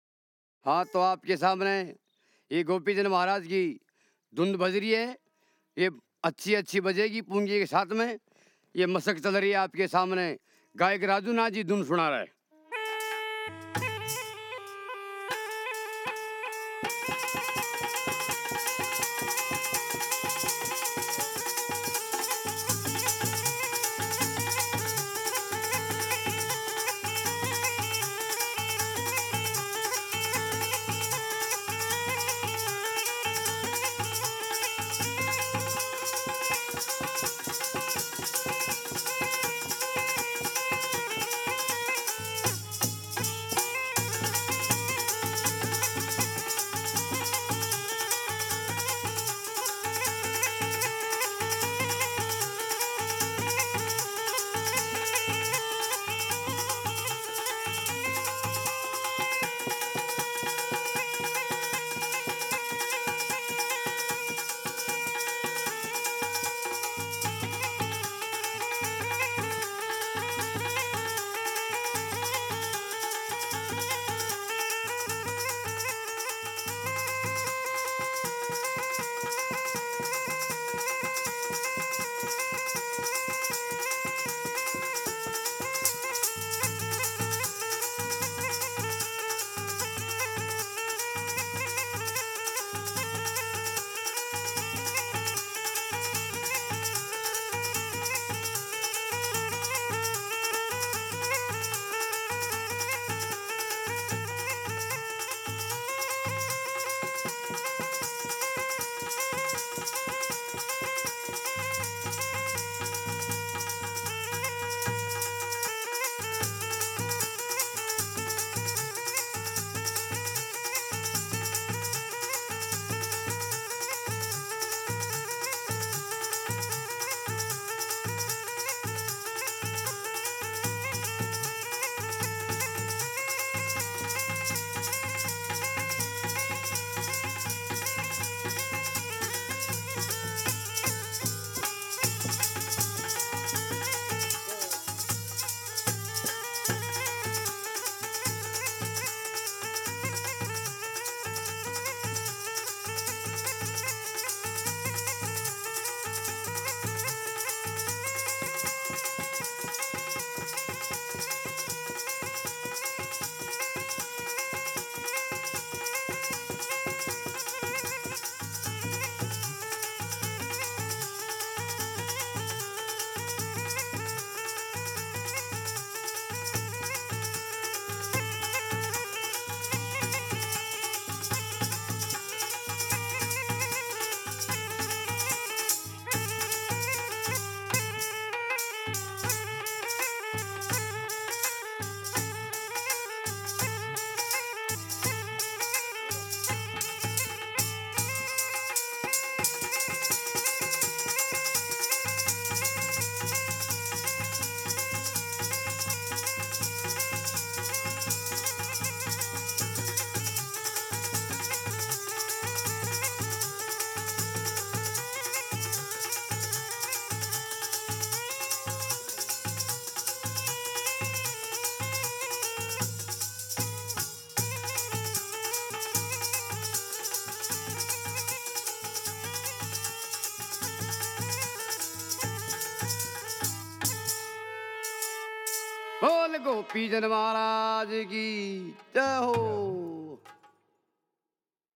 MASHAK INSTRUMENTAL - Raju Yogi & Group║BackPack Studio™ (Season 6)║Folk Music - Rajasthan
The Mashak is a Rajasthani bagpipe made from a full-size goat skin. Its bamboo blowing pipe has five finger holes, fixed in the bag with wax. Played by blowing air through the mouth, the bag serves as an air chamber controlled by the musician's arm pressure. Raju Nath & Group skillfully incorporate the hauntingly beautiful Mashak sounds, creating an immersive experience for their audience.

Song Credits -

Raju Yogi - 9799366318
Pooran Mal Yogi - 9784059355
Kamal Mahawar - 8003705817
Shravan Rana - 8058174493